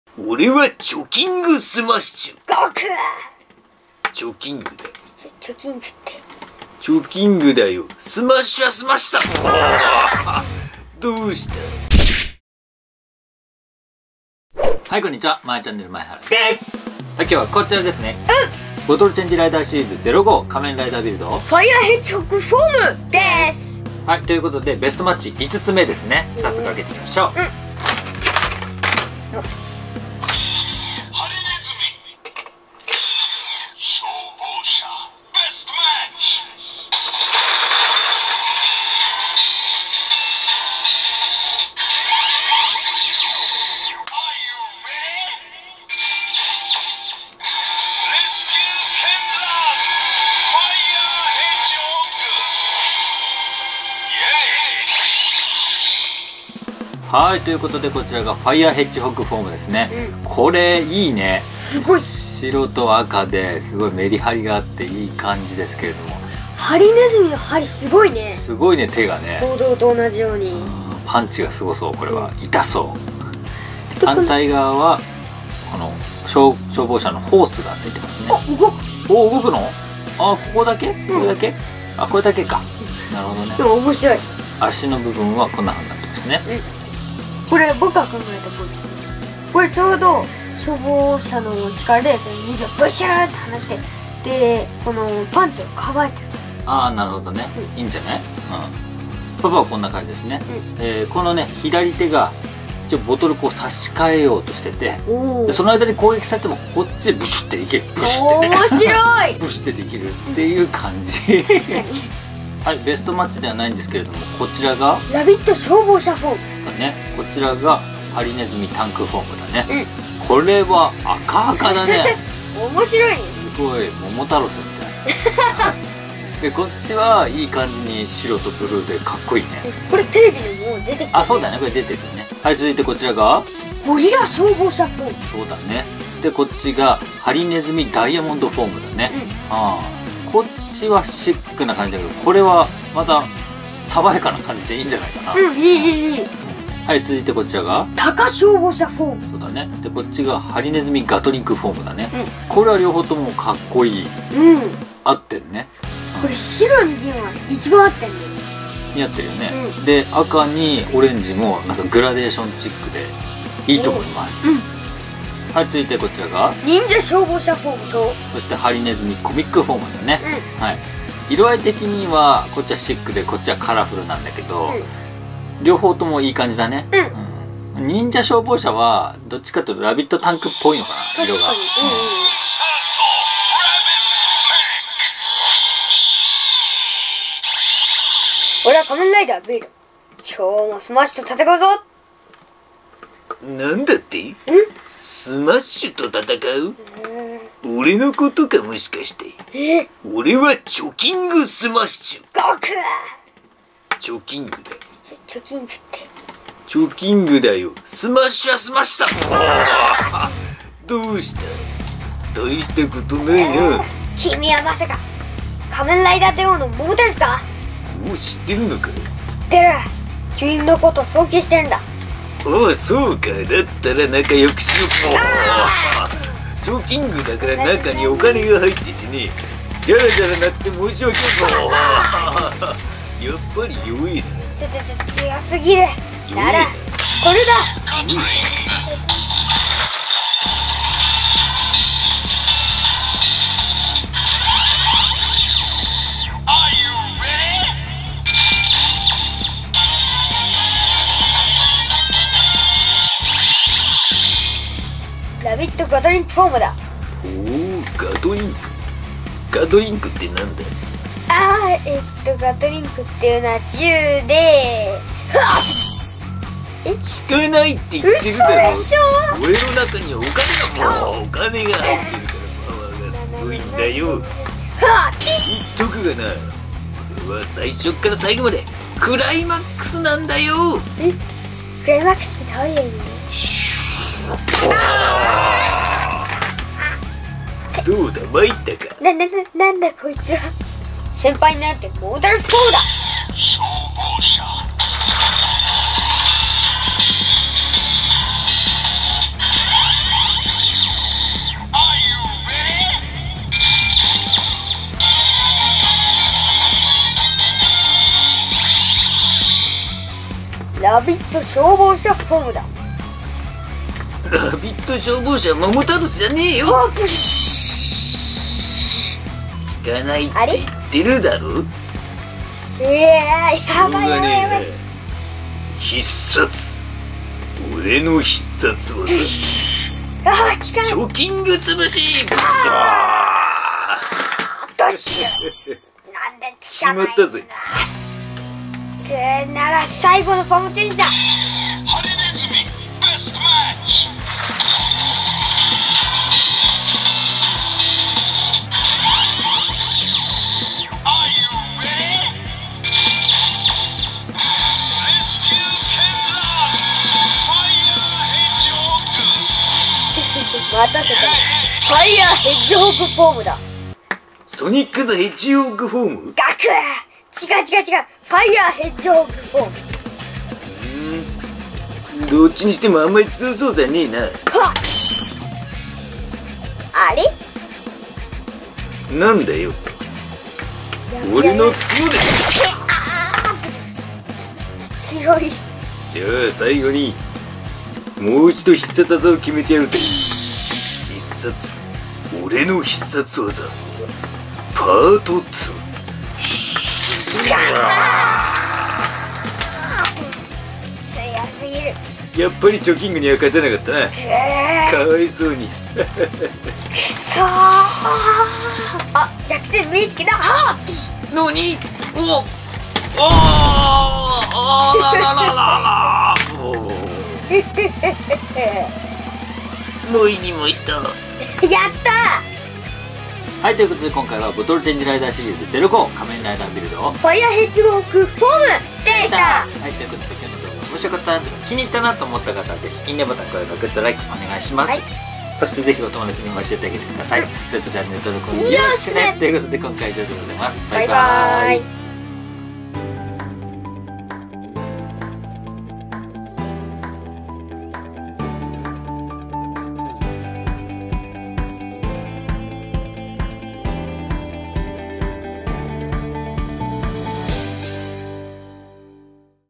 寸劇あり！ファイヤーヘッジホッグフォーム vs モモタロススマッシュ 仮面ライダービルド ボトルチェンジライダーシリーズ05 | まえちゃんねる
仮面ライダービルドの「ボトルチェンジライダーシリーズ05 ファイヤーヘッジホッグフォーム」を使って遊んでみました^^
動画の最後にはエグゼイドとの寸劇もありますので是非ご覧ください！

▽仮面ライダービルド ボトルチェンジライダーシリーズ 05仮面ライダービルド ファイヤーヘッジホッグフォーム（Amazon）